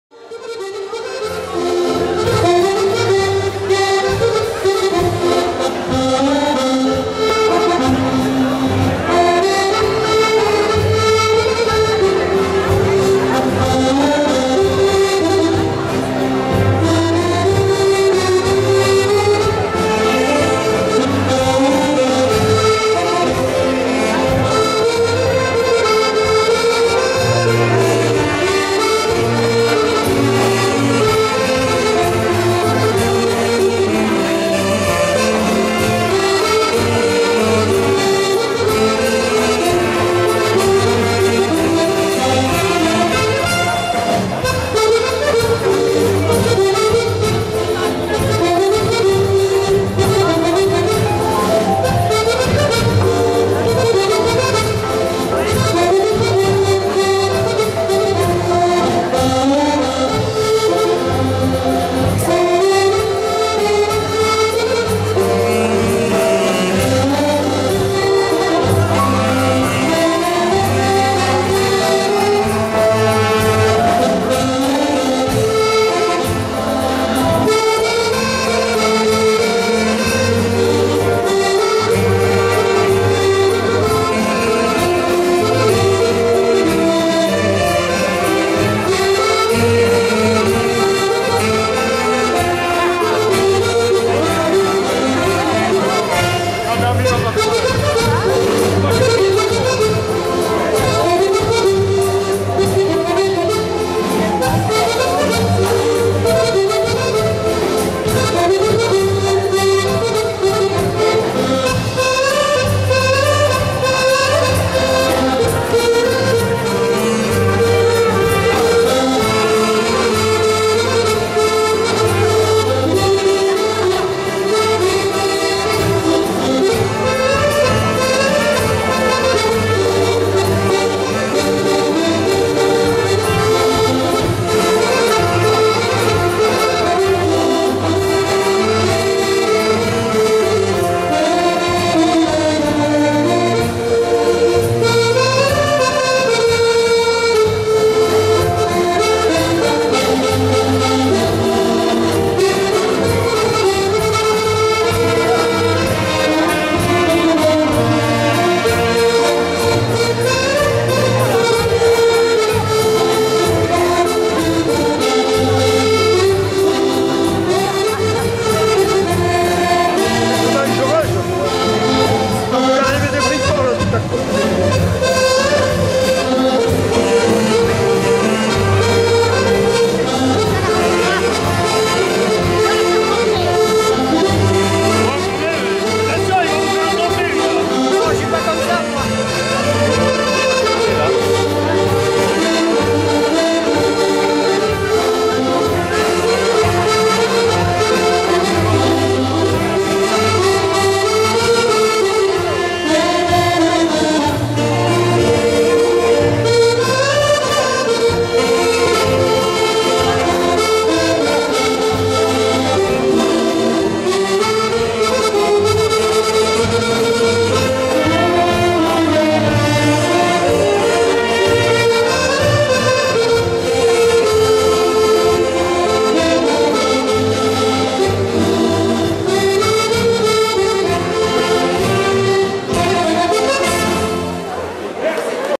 Jean François MEZY MARVEJOLS (48) mai 2014 Valse
Jean François MEZY /2e gala à Gégé à MARVEJOLS(48) le 2 mai 2014 avec l'Orchestre de Jacques GRANIE/Vidéo et réalisation Alexandra ASTORG "Sandy"